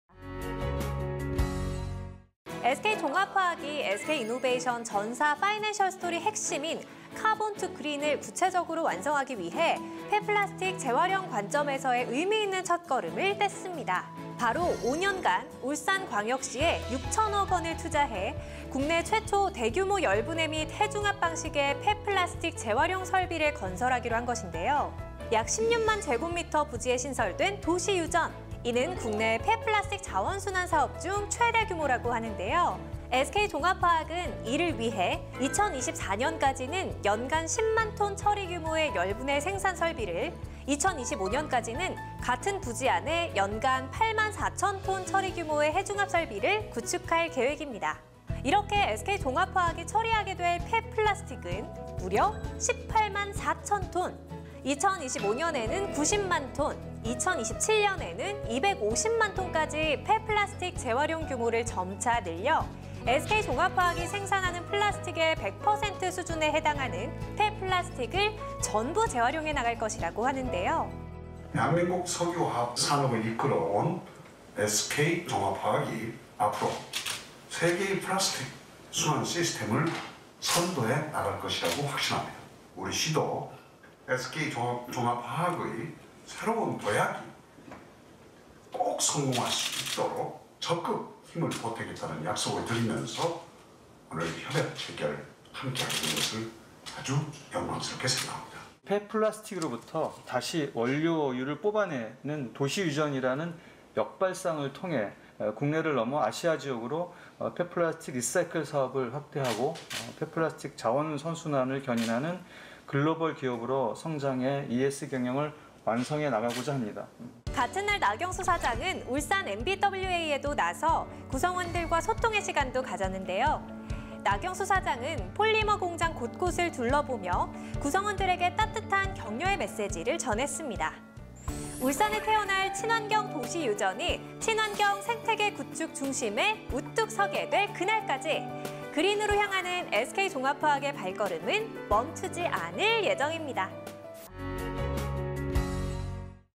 “Carbon to Green” 플라스틱 순환 시스템을 선도해 나가는 SK종합화학의 새로운 도약!
SK종합화학이 기존 석유화학 사업에서 친환경 사업으로 전환하는 ‘그린 트랜스포메이션(Green Transformation) 전략’을 본격적으로 실행하기 위해 의미 있는 첫걸음을 내딛었습니다! 😆😆

바로, 5년간 울산광역시에 6천억 원을 투자해 국내 최초! 대.규.모. 열분해 및 해중합 방식의 폐플라스틱 재활용 설비를 건설하기로 한 것!

이는 국내 폐플라스틱 자원순환 사업 중 최대 규모로, 2024년까지는 연간 10만 톤을 처리할 수 있는 열분해 생산설비를 설치할 계획인데요. 2025년까지는 같은 부지 안에 무려 연간 8만 4천 톤 처리 규모의 해중합 설비를 구축할 계획입니다! 👍

이렇게 SK종합화학이 처리하게 될 폐플라스틱은 무려 18만 4천 톤! 2027년에는 250만 톤으로 폐플라스틱 재활용 규모를 점차 늘려 SK종합화학이 생산하는 플라스틱의 100% 수준에 해당되는 폐플라스틱을 재활용해 나갈 예정입니다.

그린으로 향하는 SK종합화학의 새로운 도약! 함께 알아보러 갈까요? 👉👉

▶ 보다 자세한 소식을 SKinno News에서 확인하세요!
SK종합화학, ‘Carbon to Green’ 파이낸셜 스토리 본격 실행 – 국내 최대규모 ‘폐플라스틱 자원순환 설비, 친환경 도시유전’ 울산에 건설키로 ☞

▶ 여러분들을 위한 타임라인
친환경 도시유전 사업 투자 양해각서 체결 (00:15)
폐플락스틱 자원순환사업 규모 (00:37)
핵심 전략 첫번째, Green Anchoring (01:19)
울산광역시 송철호 시장 발표 (01:09)
SK종합화학 나경수 사장 발표 (01:37)
울산 MBWA 진행 (01:56)